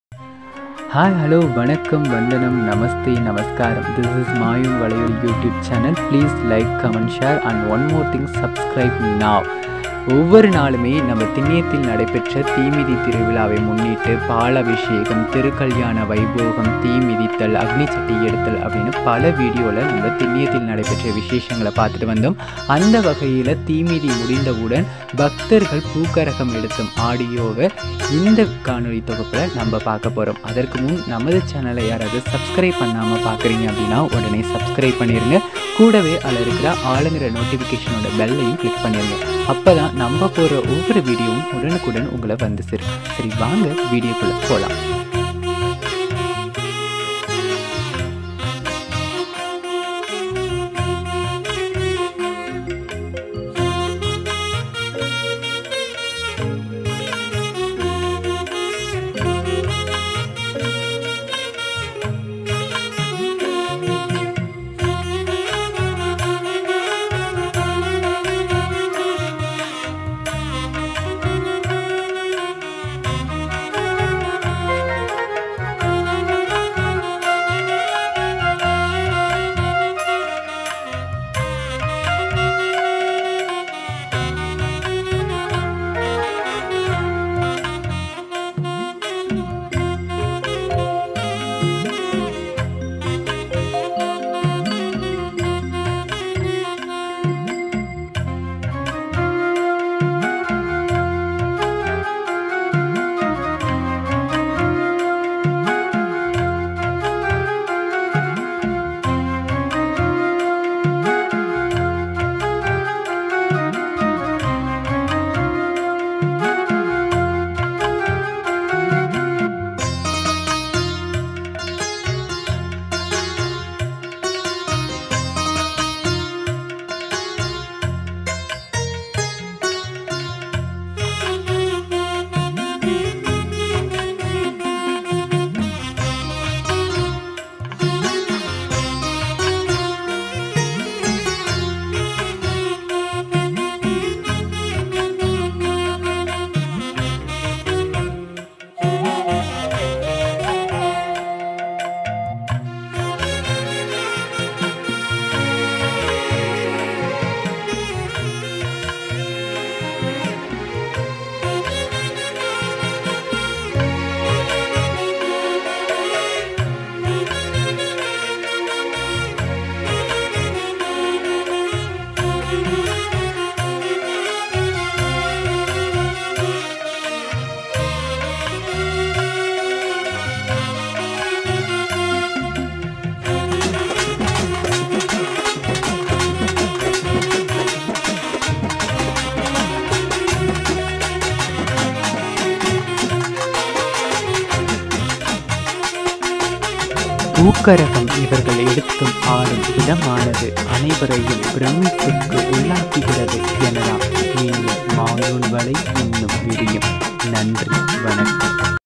பூங்கரகம் எடுத்து ஆடுதல் - திண்ணியம் திரௌபதி அம்மன் கோவில் திருவிழா | MAYOAN VALAIYOLI
THIS IS ABOUT POONGARAGAM DANCE IN THROUBATHI AMMAN TEMPLE FESTIVAL.